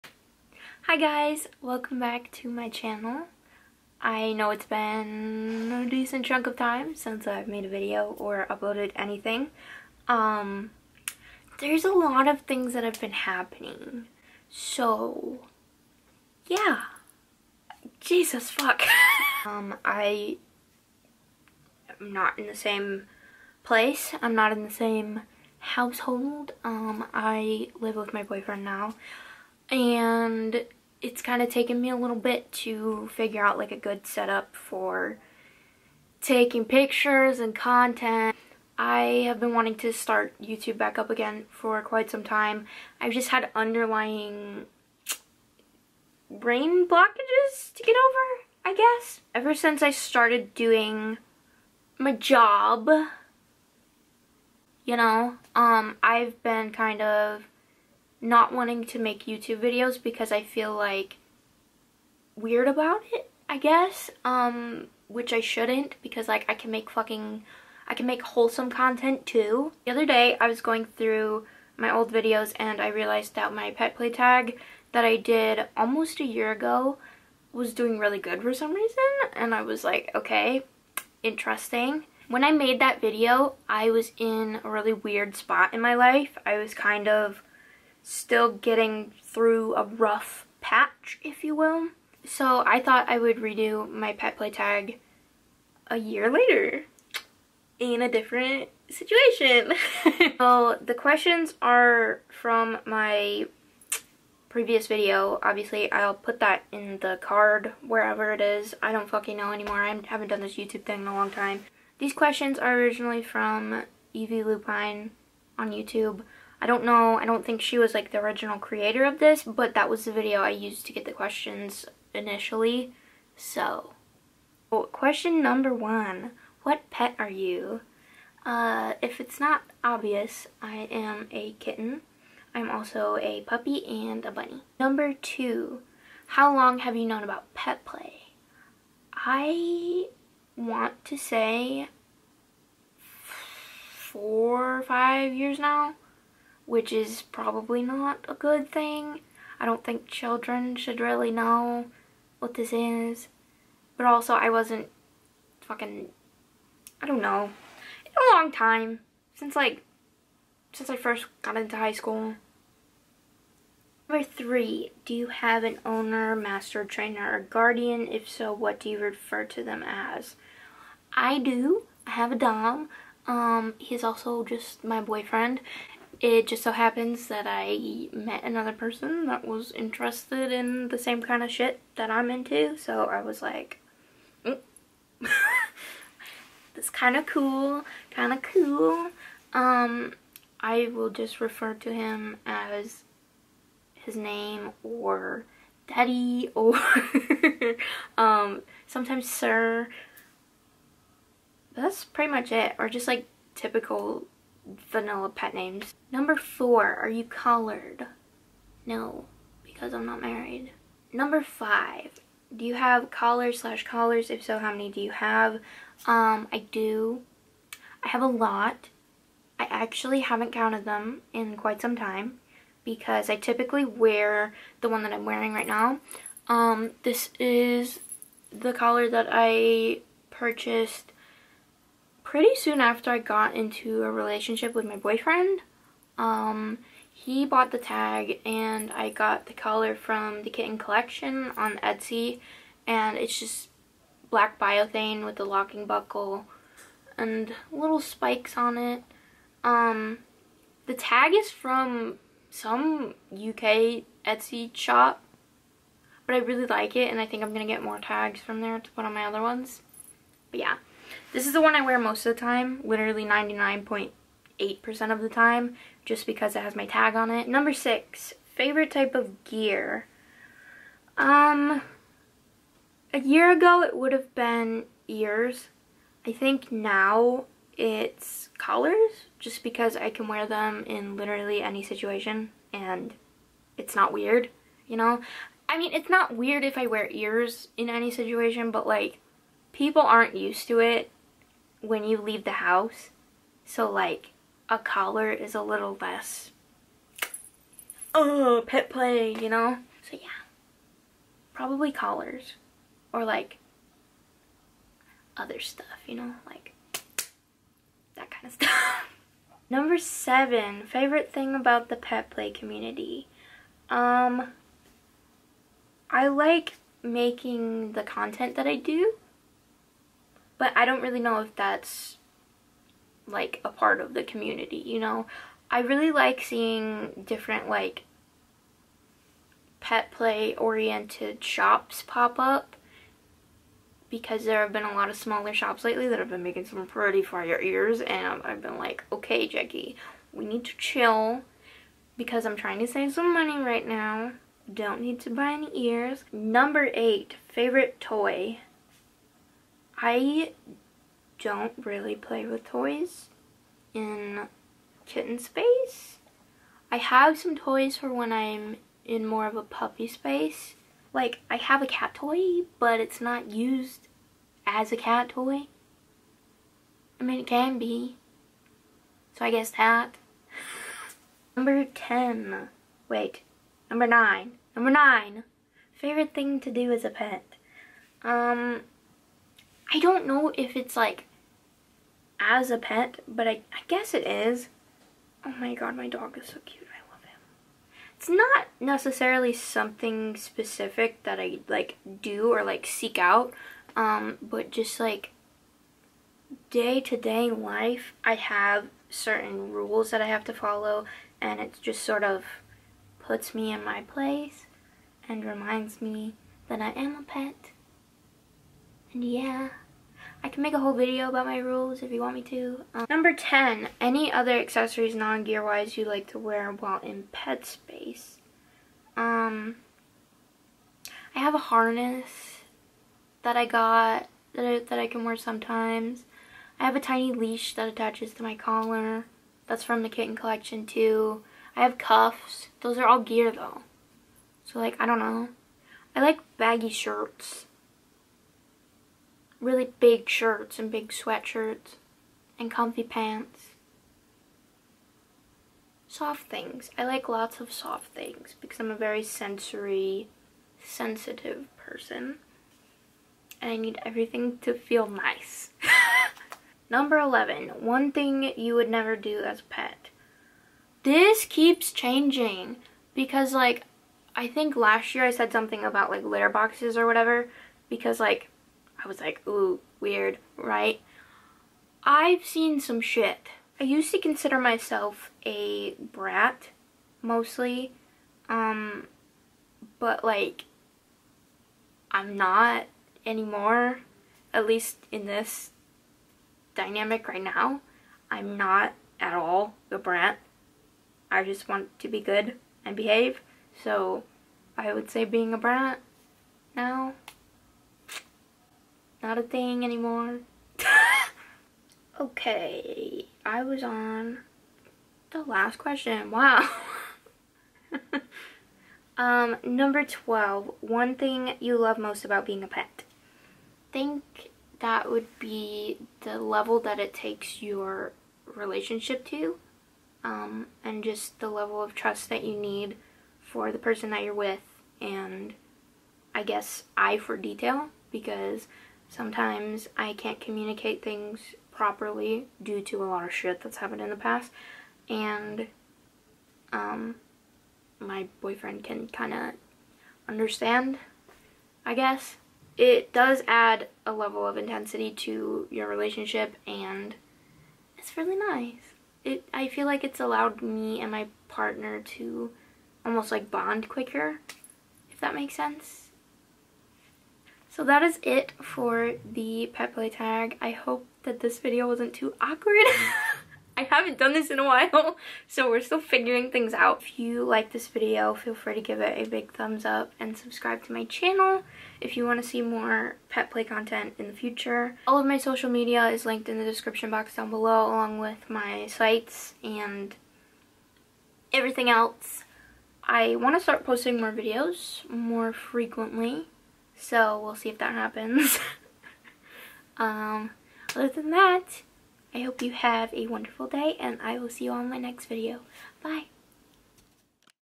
The Pet Play Tag (1 Year Later)
hi hi! it's been a minute! today i'm doing the petplay tag again, one year later. a lot has changed since i made the initial video so i thought it would be cute to do it again in a completely different place in my life. hope you enjoy!